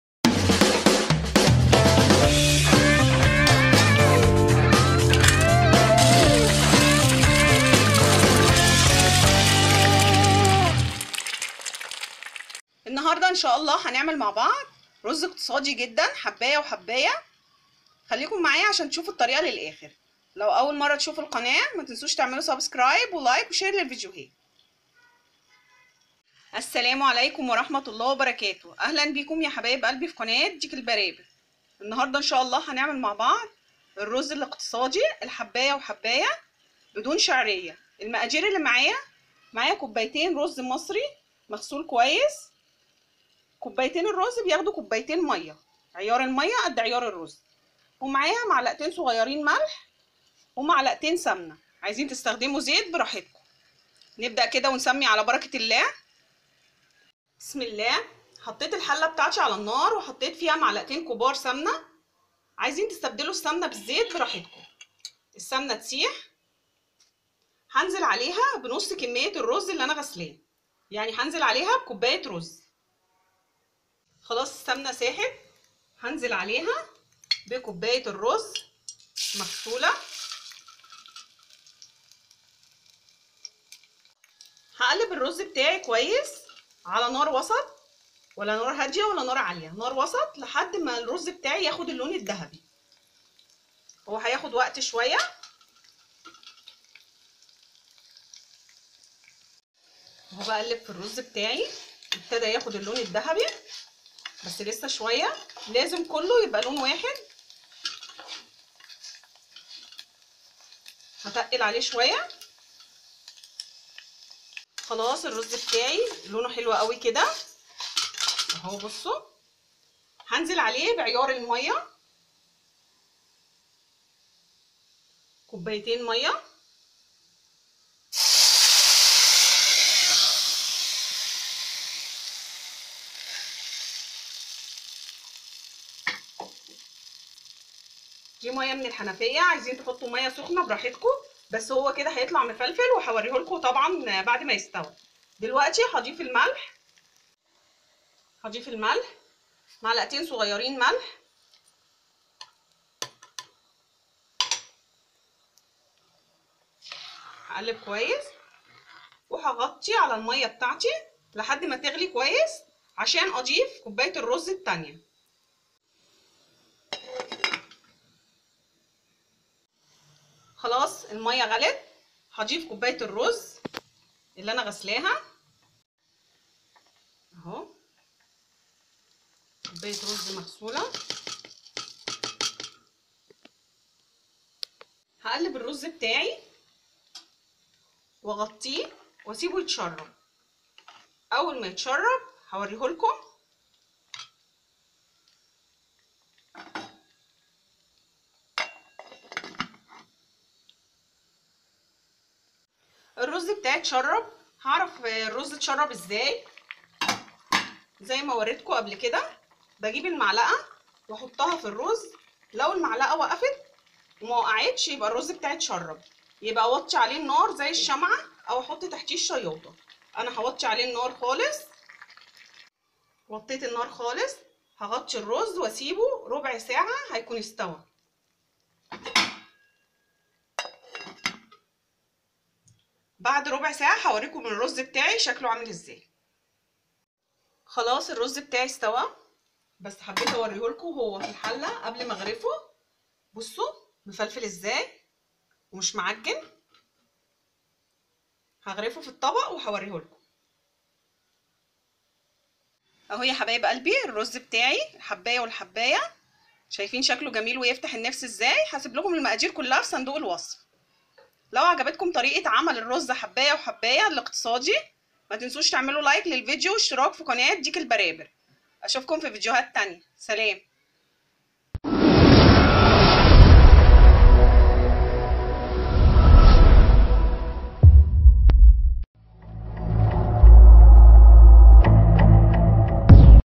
أسهل طريقة لعمل الأرز حباية وحباية الاقتصادي
اتنين كوب أرز مصري مغسول
اتنين كوب ماء
ملعقتين كبار سمنه
ملعقتين صغار ملح